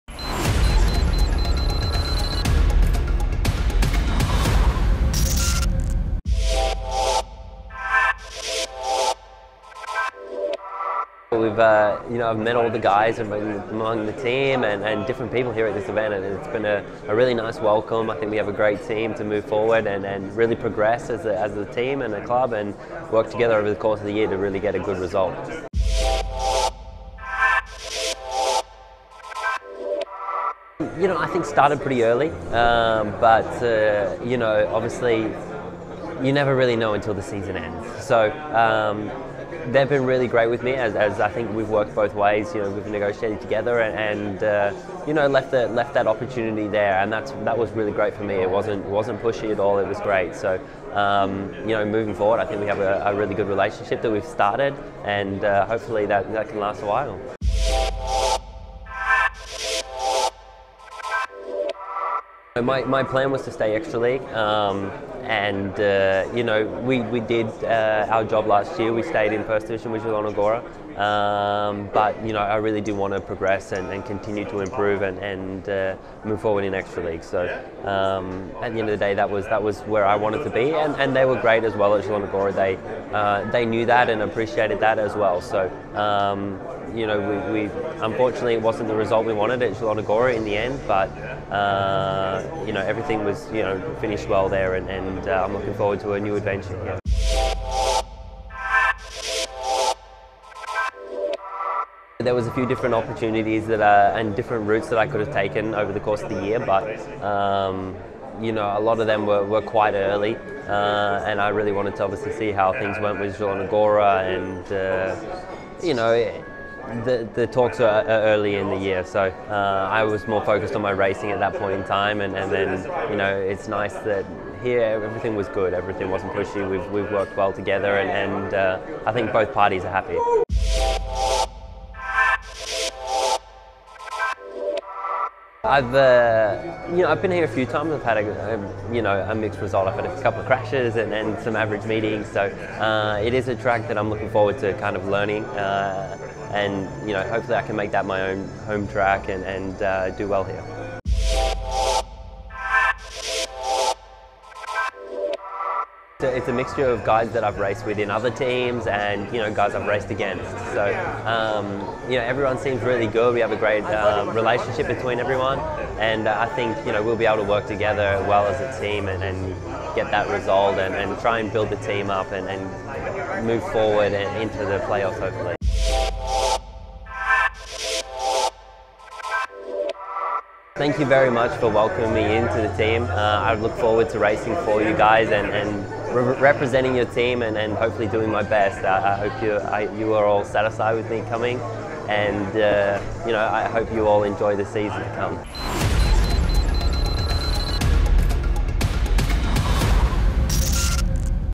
Max Fricke o powodach powrotu do PGE Ekstraligi
- Nawiązaliśmy dobre relacje i myślę, że zostanę tutaj na dłużej - mówi nowy zawodnik ZOOLESZCZ GKM Grudziądz, Max Fricke. - Chcę robić postępy i dlatego wróciłem do PGE Ekstraligi - dodaje Australijczyk.